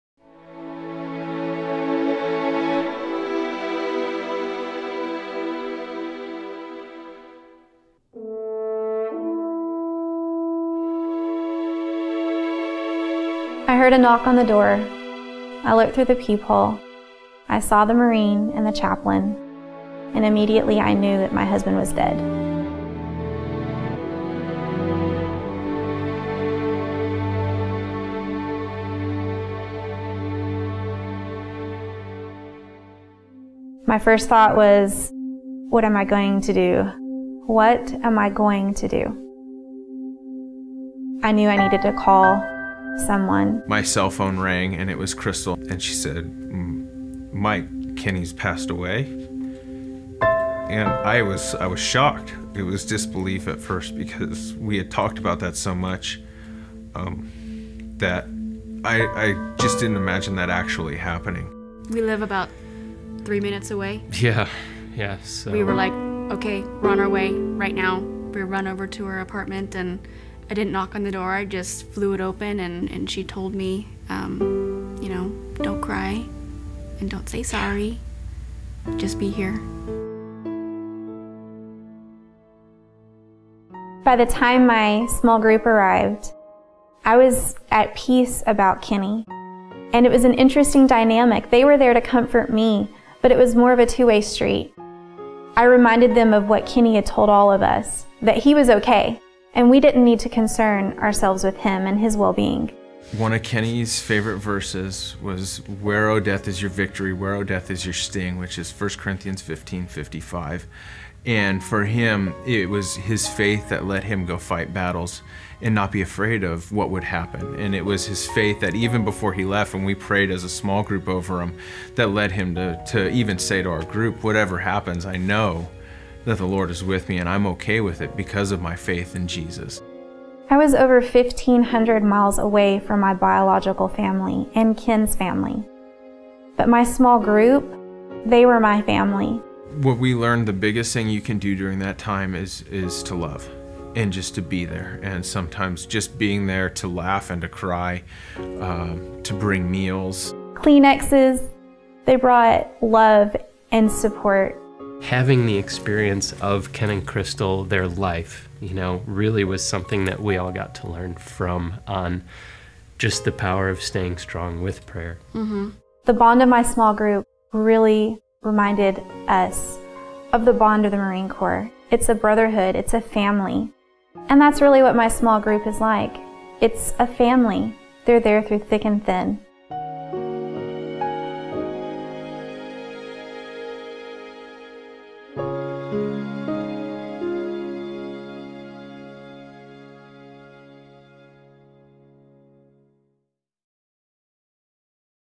Krystal May
A story about how Krystal's small group rallied around her during tragedy.